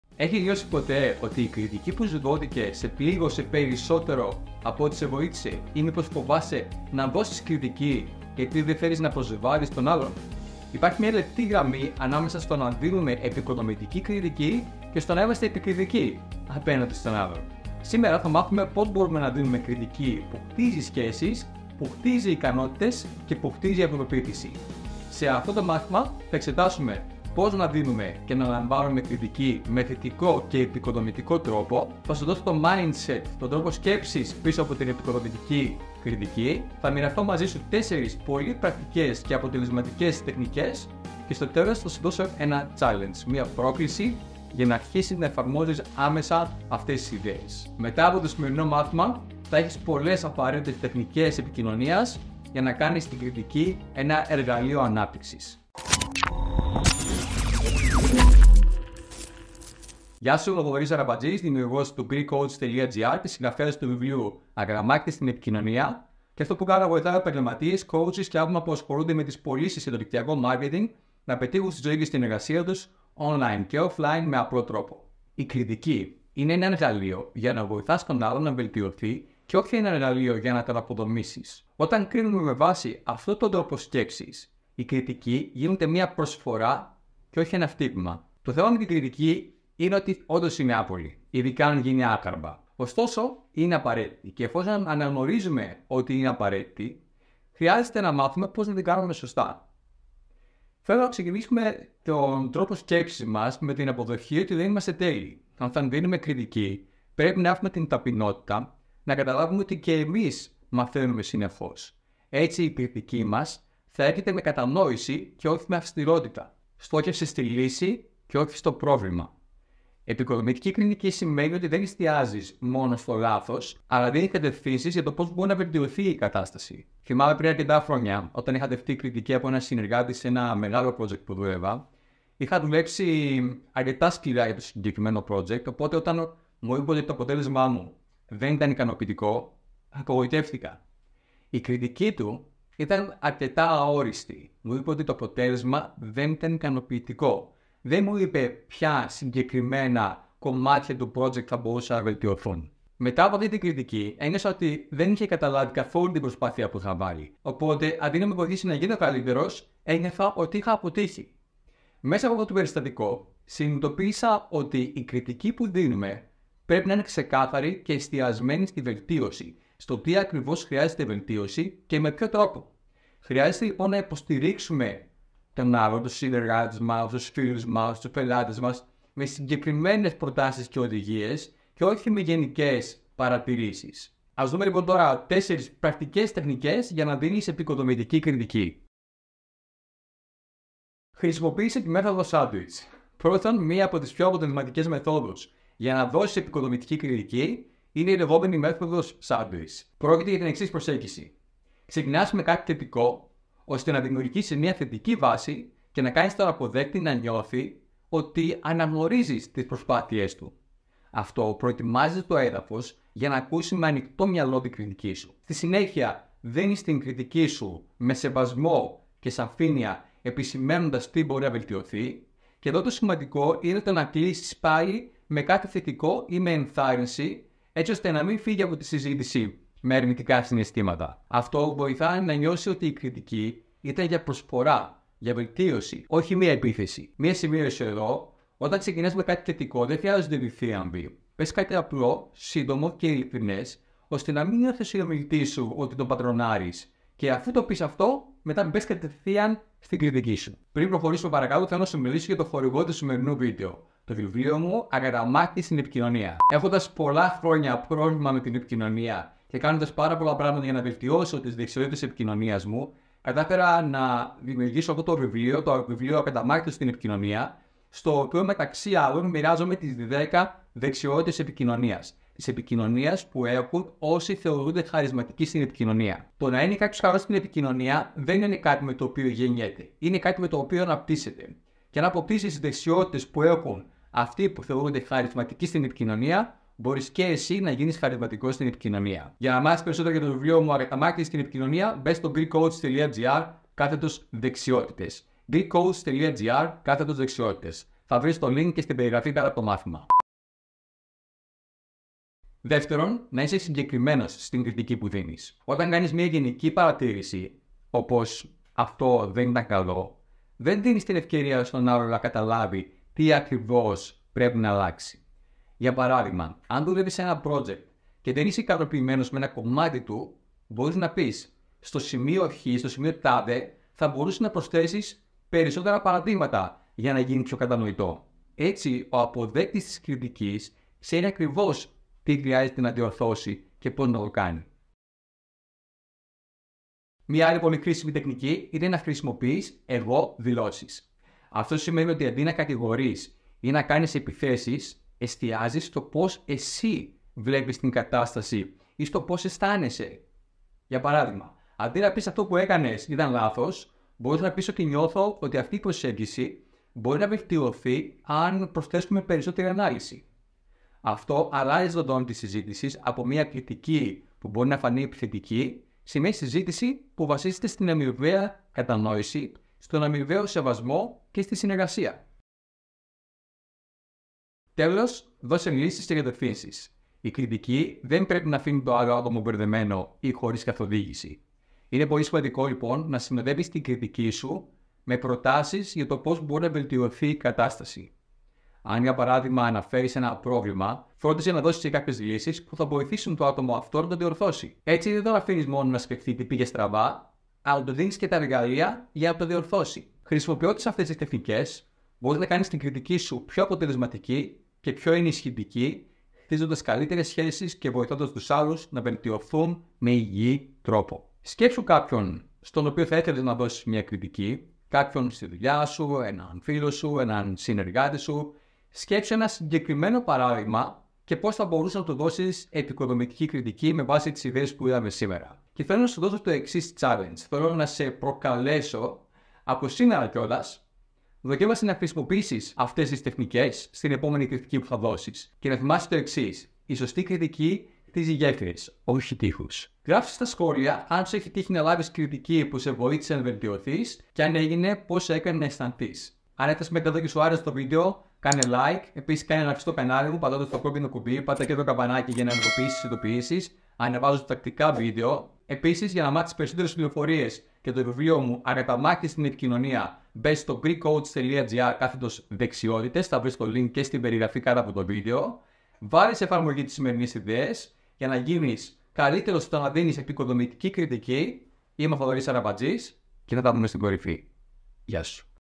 4 Τεχνικές για το πώς να δίνεις και να λαμβάνεις εποικοδομητική κριτική
Θέλεις να μάθεις πώς να δίνεις κριτική που χτίζει αντί να καταστρέφει; Σε αυτό το βίντεο, μοιράζομαι τεχνικές και στρατηγικές για να δώσεις και να λάβεις εποικοδομητική κριτική που οδηγεί στην ανάπτυξη και όχι στην απογοήτευση.

Ανακάλυψε πώς μπορείς να βοηθήσεις τους άλλους να βελτιωθούν, χωρίς να πληγώνεις την αυτοπεποίθησή τους. Δες πώς μια σωστή προσέγγιση στην κριτική μπορεί να κάνει τη διαφορά σε κάθε σχέση – επαγγελματική ή προσωπική!